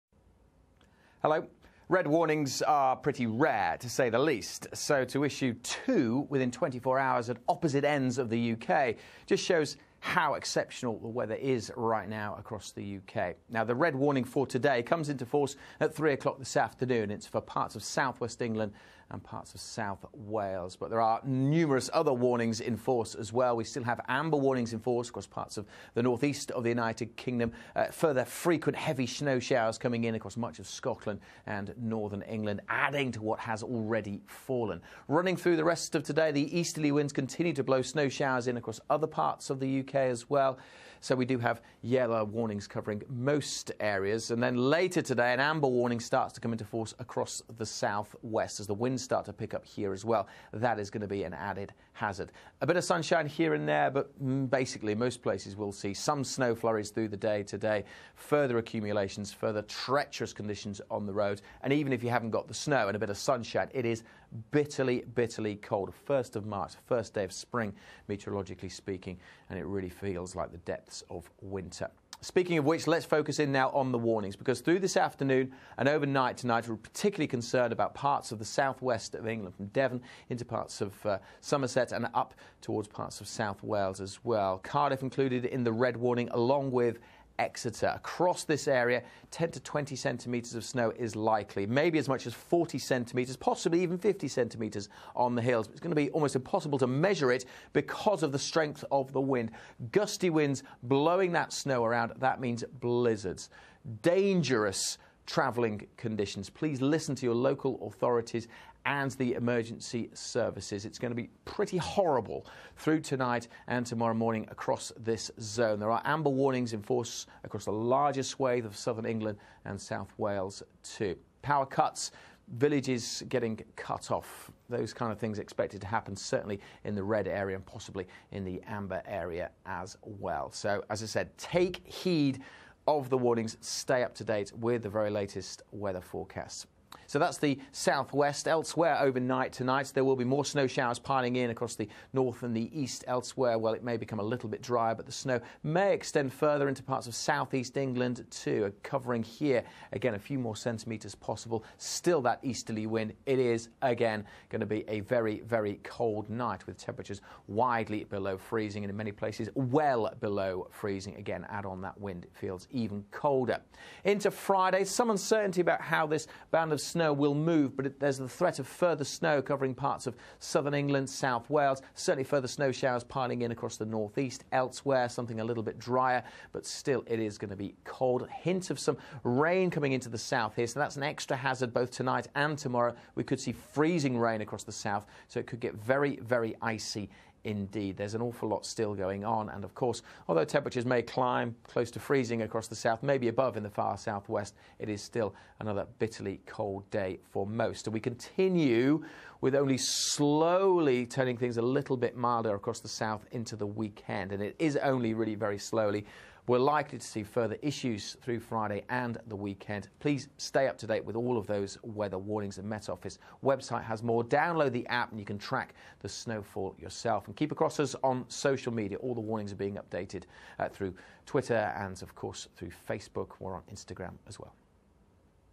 Storm Emma to smash UK (Latest)
SUMMERY: The Met Office issues a red 'danger to life' weather warning for south-west England and south Wales, from 15:00. Amber warnings are in place in Scotland, northern and eastern England, and the East Midlands National Grid warns it may not have enough gas to meet UK demand on Thursday.
Hundreds of people were stranded on the M80 in Scotland overnight.
The south-west of England, Wales and Northern Ireland are bracing themselves for snow as Storm Emma makes its way up from France.
Rail and air travel will be disrupted for a third day and some rural communities could be cut off.